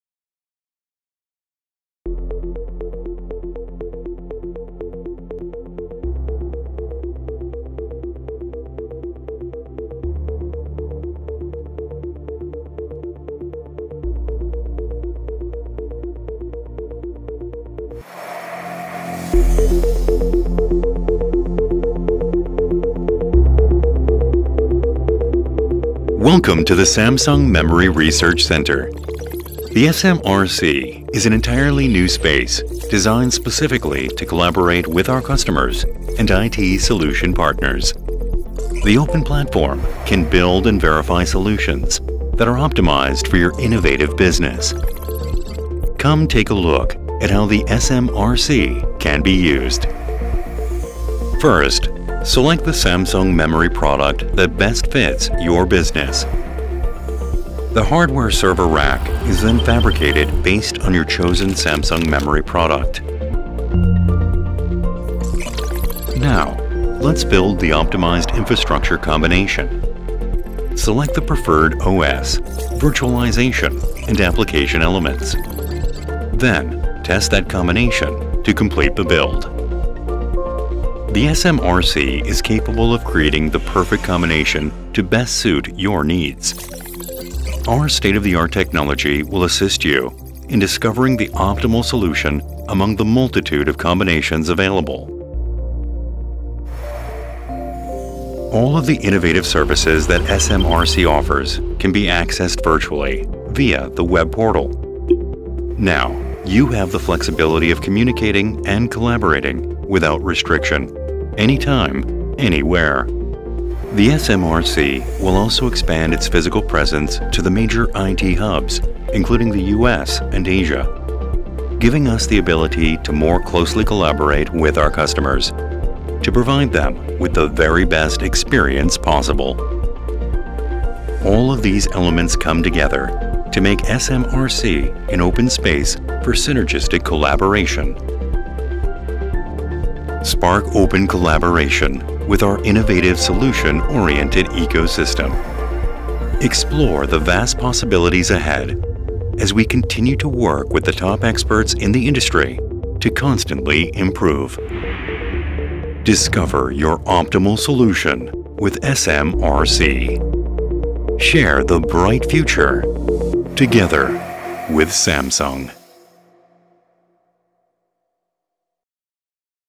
SAMSUNG | CES2023 SMRC Highlight zone
2023

CES2023 삼성반도체 사업부 전시장의 SMRC Highlight zone 컨텐츠를 제작하였습니다.
방문하는 관람객들에게 강한 인상을 남기기 위해 아나몰픽 기법을 활용하였고
SMRC라는 새로운 개념을 효과적으로 전달하기 위한 다양한 표현들을 사용하였습니다.

We produced SMRC Highlight Zone contents of CES2023 Samsung Semiconductor Business Department Exhibition Hall.
Anamorphic techniques were used to impress visitors.
Various expressions were used to effectively convey the new concept of SMRC.